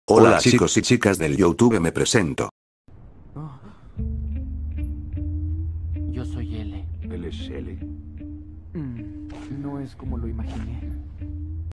Intro para mi canal
Hola chicos y chicas me presento soy L y este vídeo es una intro para mis vídeos espero y les guste :)